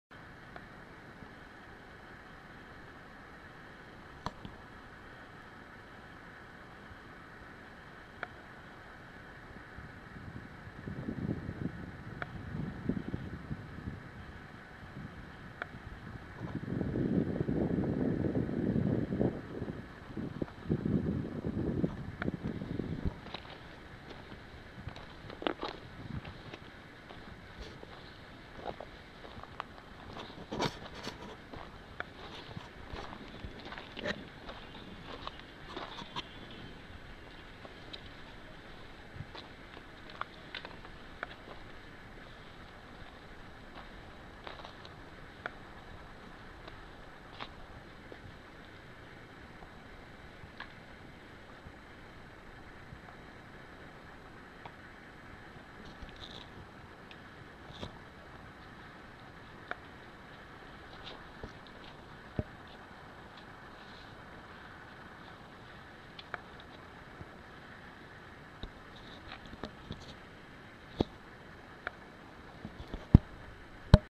Grog Pit Camp, Sumner Wash, Grand Canyon, AZ
A great place to camp below the Clear Creek Trail, Zoroaster Temple, and the redwall crack.